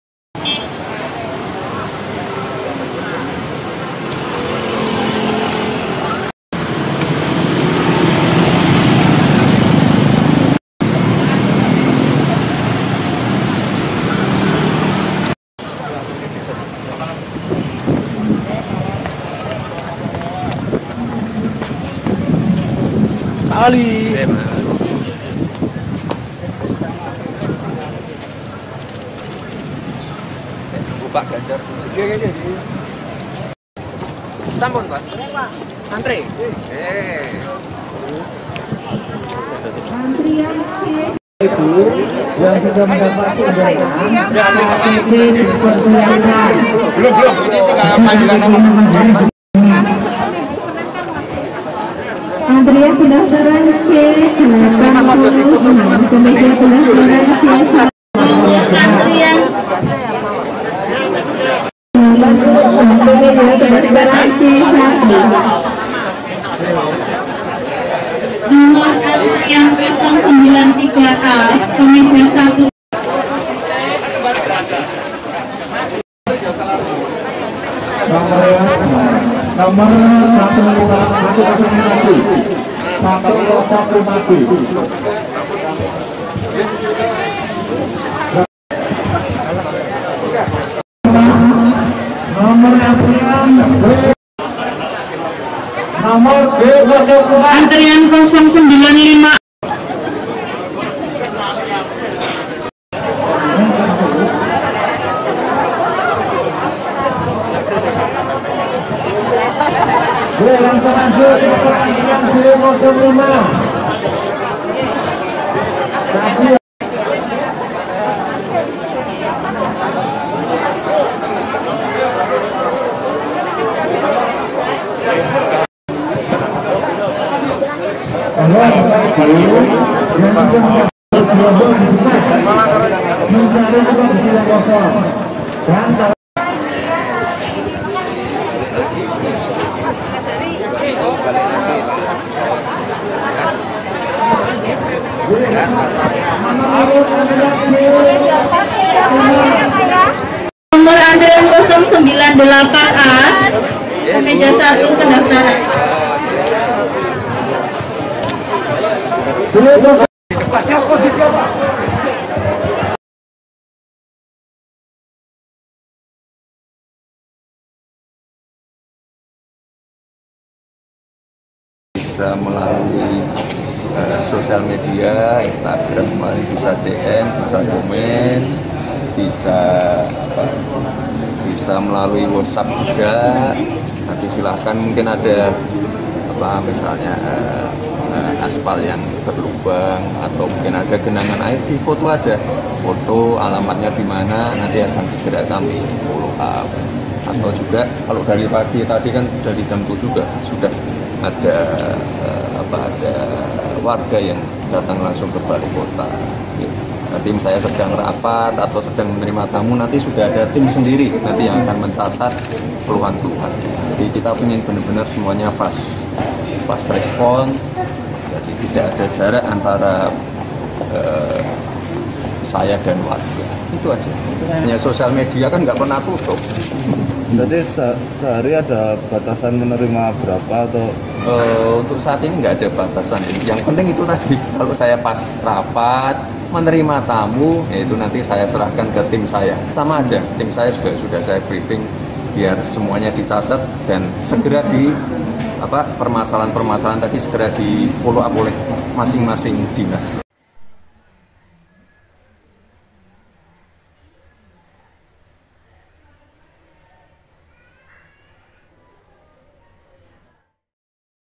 Gibran Buka Layanan Aduan untuk Warga Mayarakat
Layanan aduan yang dinamai 'Lapor Mas Wali!' tersebut beroperasi selama 24 jam non stop dan belum ada pembatasan kuota aduan per harinya.Masyarakat bisa menghubungi nomor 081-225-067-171.